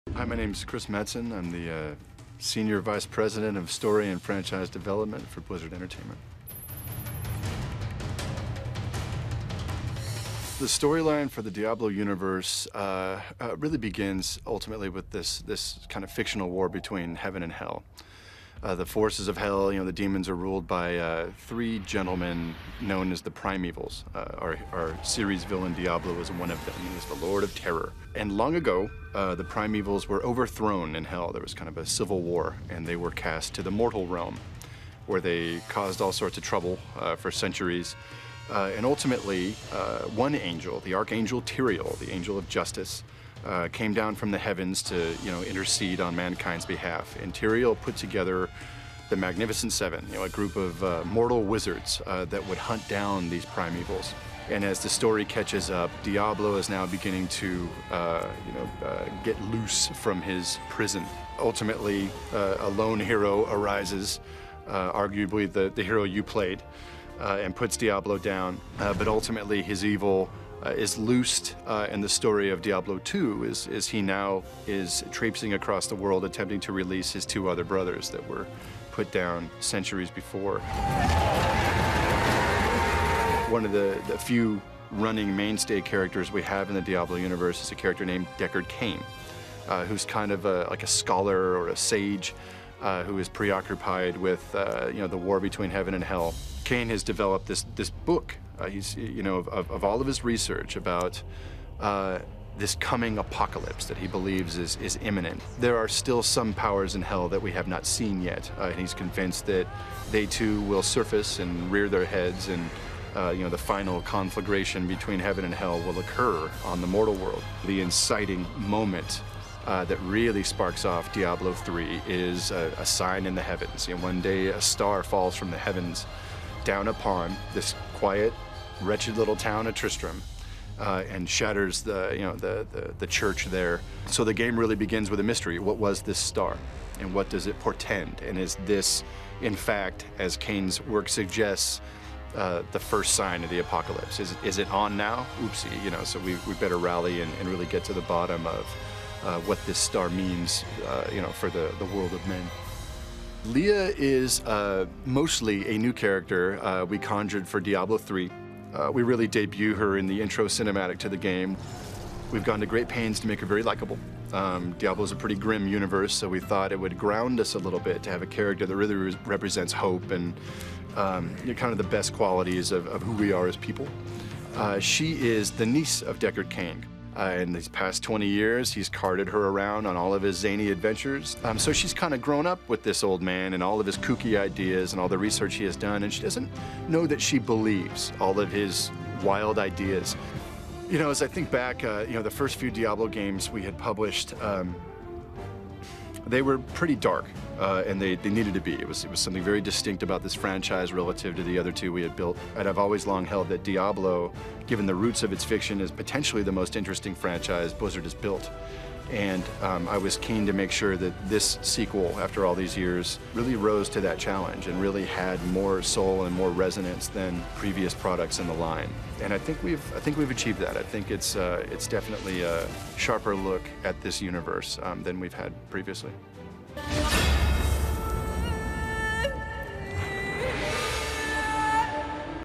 Diablo 3 Lore - Introduction to Diablo 3 - Chris Metzen - BlizzCon 2011
Chris Metzen provides us with a concise introduction to Diablo 3 through its preceding lore. A very interesting and engaging presentation with excellent accompanying imagery! If your a fan of Diablo 3, this is a must watch!